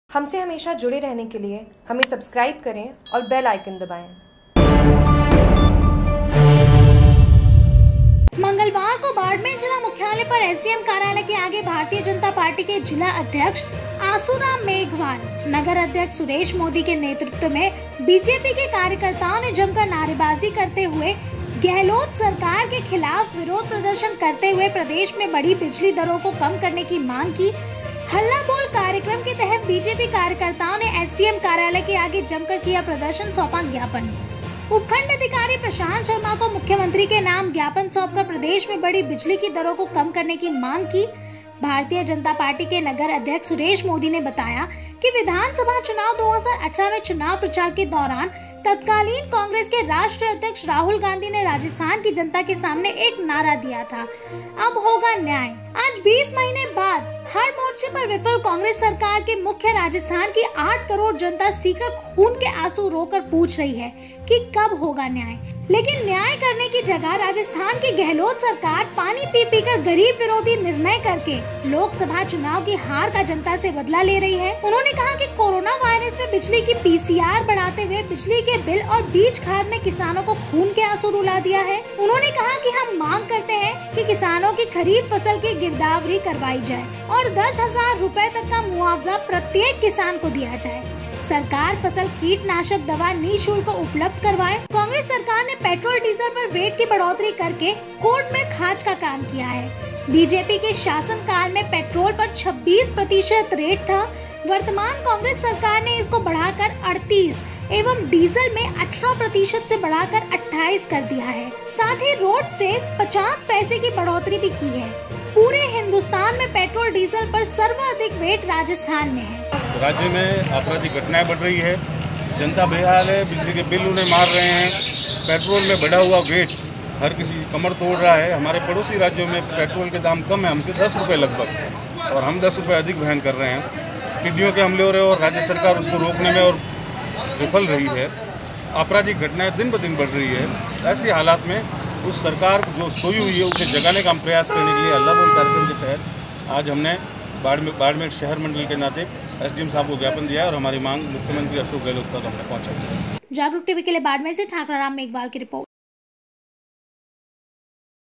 Jagruk TV : बीजेपी के कार्यकर्ताओं ने जमकर नारेबाजी करते हुए, गहलोत सरकार के खिलाफ विरोध प्रदर्शन
मंगलवार को बाड़मेर जिला मुख्यालय पर एसडीएम कार्यालय के आगे भारतीय जनता पार्टी के जिला अध्यक्ष आसुराम मेघवाल नगर अध्यक्ष सुरेश मोदी के नेतृत्व में बीजेपी के कार्यकर्ताओं ने जमकर नारेबाजी करते हुए गहलोत सरकार के खिलाफ विरोध प्रदर्शन करते हुए प्रदेश में बड़ी बिजली की दरों को कम करने की मांग की हल्ला बोल कार्यक्रम के तहत बीजेपी कार्यकर्ताओं ने एसडीएम कार्यालय के आगे जमकर किया प्रदर्शन सौंपा ज्ञापन उपखंड अधिकारी प्रशांत शर्मा को मुख्यमंत्री के नाम ज्ञापन सौंपकर प्रदेश में बड़ी बिजली की दरों को कम करने की मांग की भारतीय जनता पार्टी के नगर अध्यक्ष सुरेश मोदी ने बताया कि विधानसभा चुनाव 2018 में चुनाव प्रचार के दौरान तत्कालीन कांग्रेस के राष्ट्रीय अध्यक्ष राहुल गांधी ने राजस्थान की जनता के सामने एक नारा दिया था अब होगा न्याय आज 20 महीने बाद हर मोर्चे पर विफल कांग्रेस सरकार के मुखिया राजस्थान की आठ करोड़ जनता सीकर खून के आंसू रो कर पूछ रही है कि कब होगा न्याय लेकिन न्याय करने की  जगह राजस्थान की गहलोत सरकार पानी पी पीकर गरीब विरोधी निर्णय करके लोकसभा चुनाव की हार का जनता से बदला ले रही है उन्होंने कहा कि कोरोनावायरस में बिजली की पीसीआर बढ़ाते हुए बिजली के बिल और बीज खाद में किसानों को खून के आंसू रुला दिया है उन्होंने कहा कि हम मांग करते हैं कि किसानों की खरीफ फसल की गिरदावरी करवाई जाए और ₹10000 तक का मुआवजा प्रत्येक किसान को दिया जाए सरकार फसल कीटनाशक दवा निशुल्क उपलब्ध करवाएं कांग्रेस सरकार ने पेट्रोल डीजल पर वेट की बढ़ोतरी करके कोड में खाज का काम किया है भाजपा के शासनकाल में पेट्रोल पर 26% रेट था वर्तमान कांग्रेस सरकार ने इस को बढ़ाकर 38% एवं डीजल में 18% से बढ़ाकर 28% कर दिया है साथ ही रोड सेस 50 पैसे की बढ़ोतरी कर भी है पूरे हिंदुस्तान में पेट्रोल डीजल पर सर्वाधिक वेट राजस्थान में है

Jagruk TV